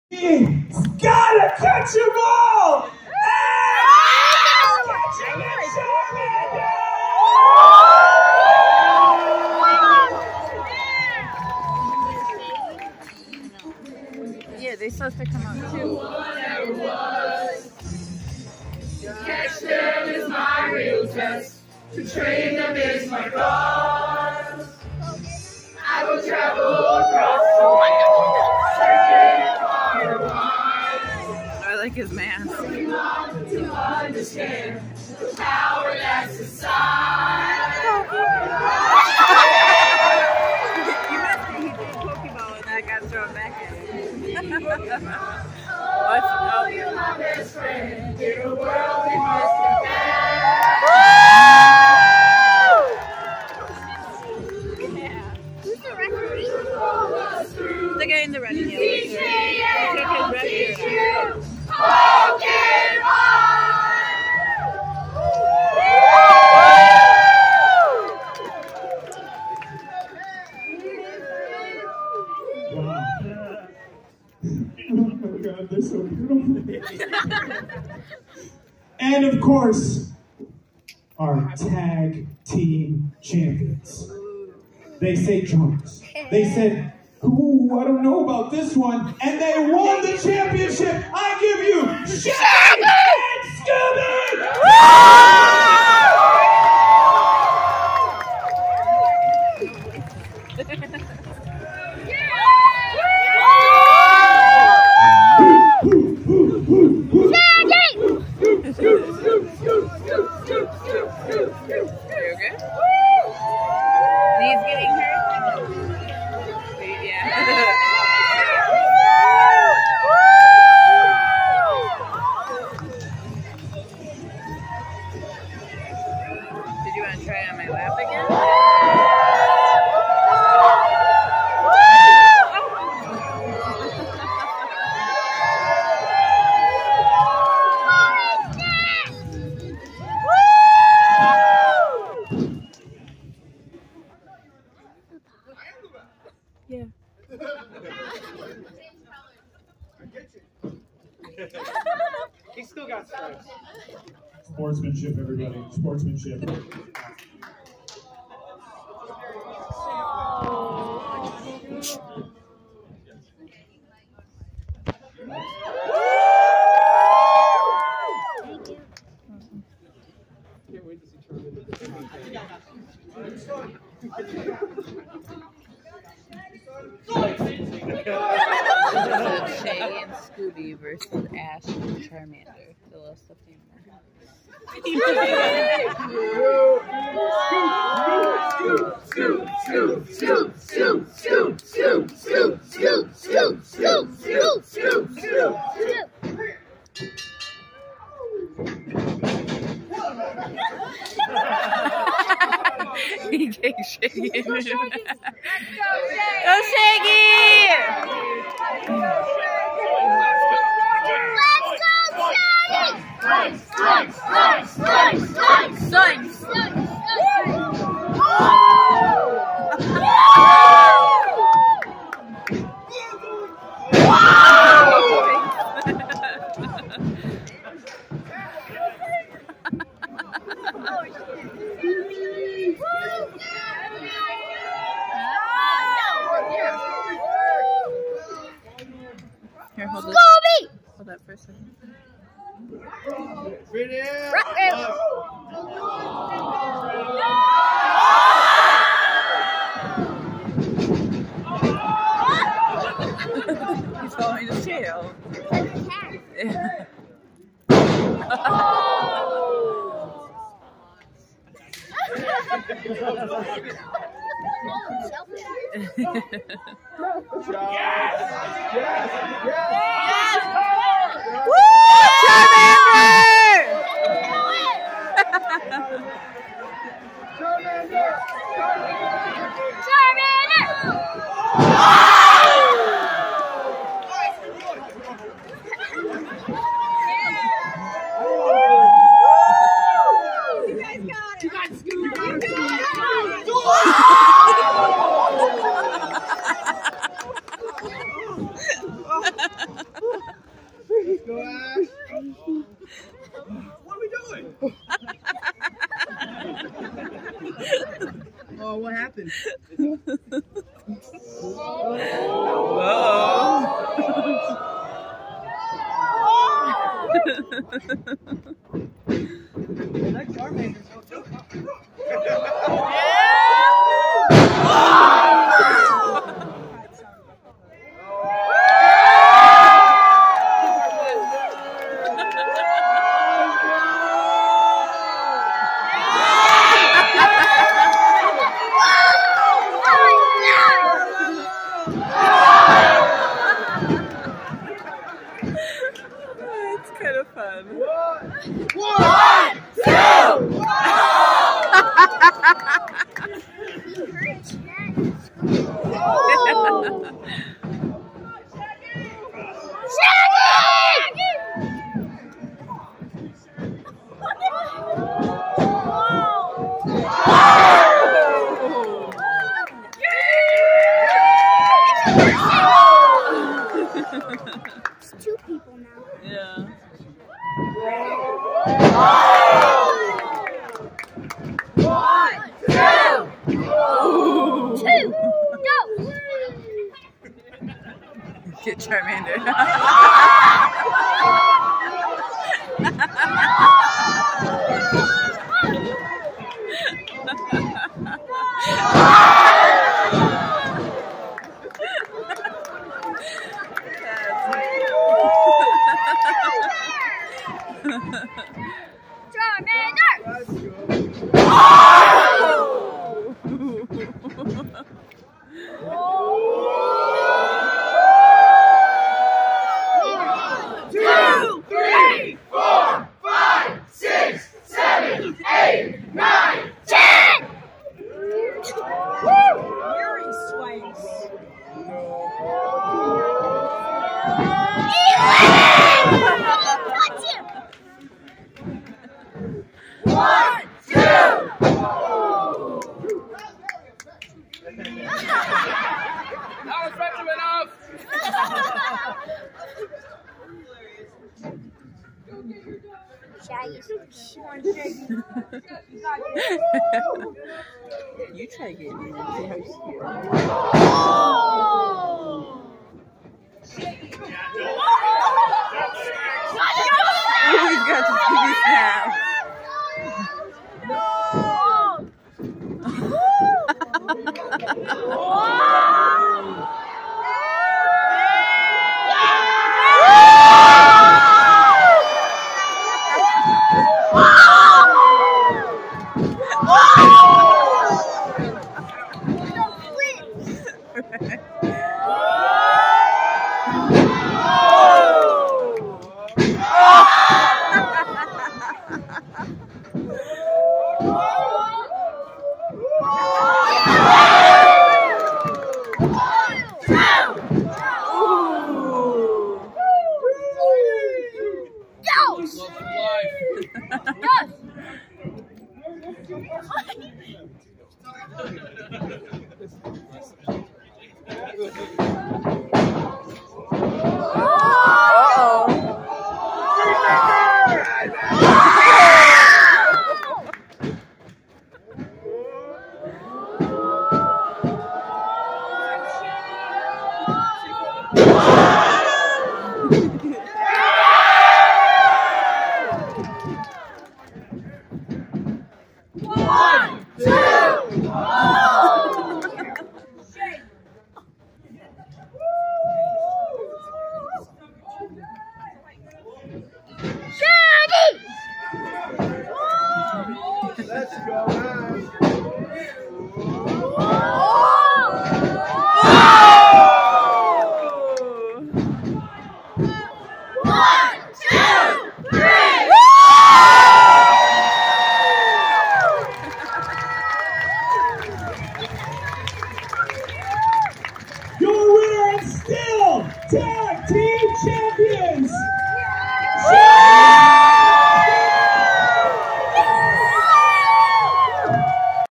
Ash and Charmander vs Shaggy and Scooby!
What a great tag team match that was! I can't wait to attend another maximum power event!